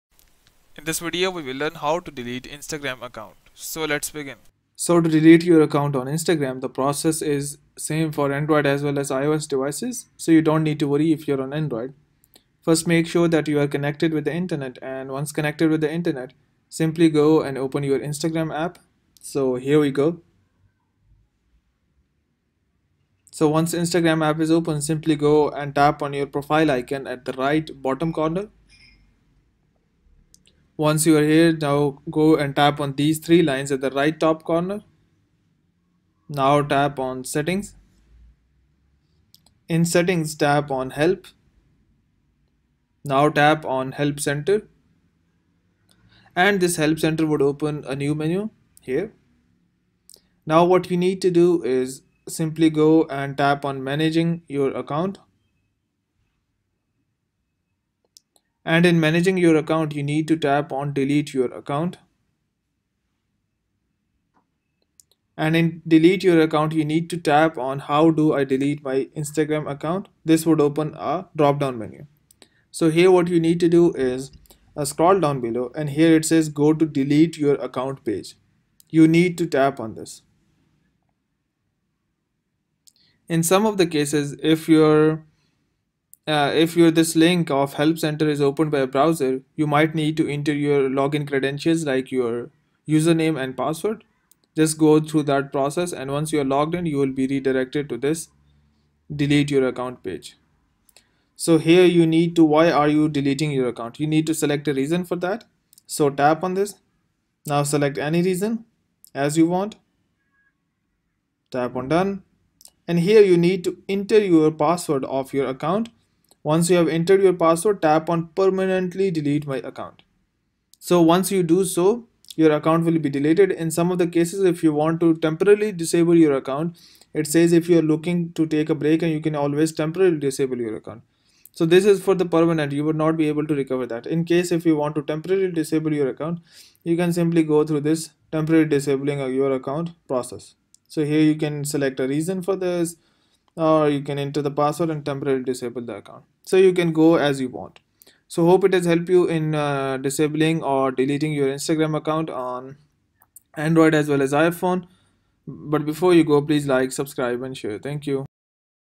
2021 如何永久删除 Instagram 帐户 | 删除 Instagram 帐户
了解如何删除 Instagram (ig) 帐户 iPhone 和 Android。在本视频中，您将完成删除 Instagram id 和删除 Instagram 个人资料的整个过程。您还可以通过此过程永久删除 Instagram 帐户。